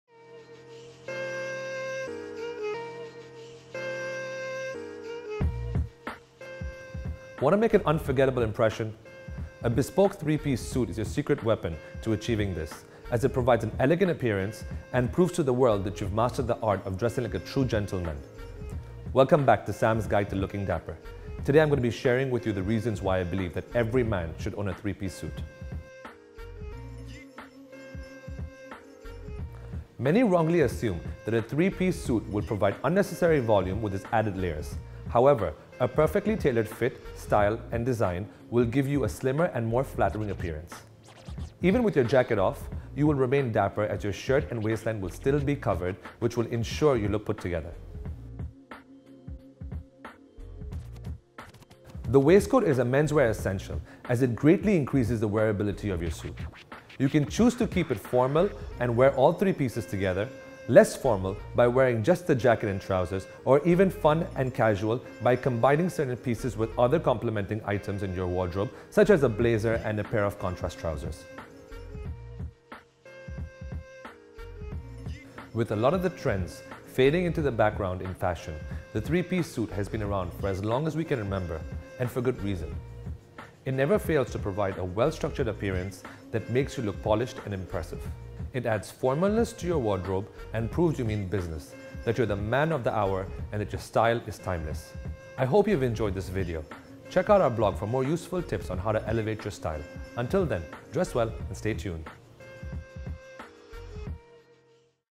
Sam's Guide Episode 8: Three Piece Suit
The three piece suit remains an unstoppable form of timeless style, and you’re about to find out the reason as to why!